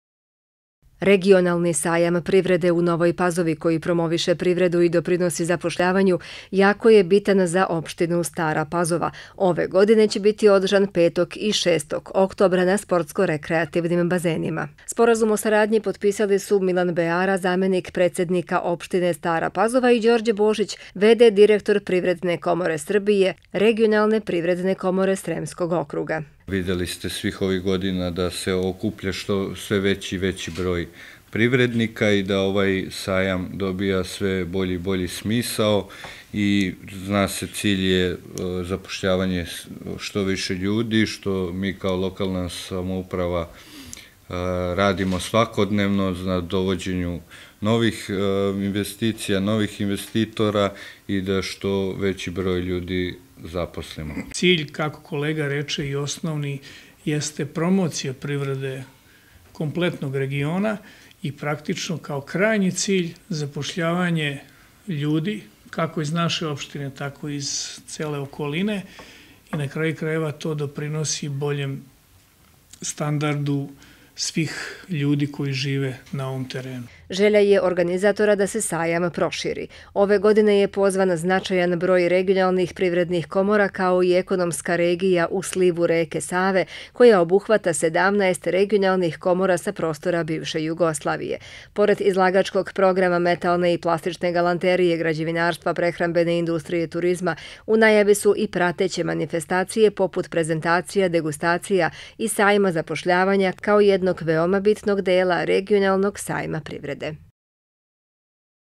Sporazum o organizaciji Sajma privrede
U zgradi Opštine Stara Pazova danas je potpisan Sporazum o saradnji  između Opštine i Privredne komore Srbije, o zajedničkoj  organizaciji Regionalnog sajma privrede. Po 11. Put ova će manifestacija biti održana na sportsko rekreativnim bazenima u Novoj Pazovi 5. i. 6. Oktobra. Ugovor su potpisali Milan Beara, zamenik predsednika Opštine i Đorđe Božić, v.d. direktor Privredne komore Srbije, Regionalne privredne komore Sremskog upravnog okruga.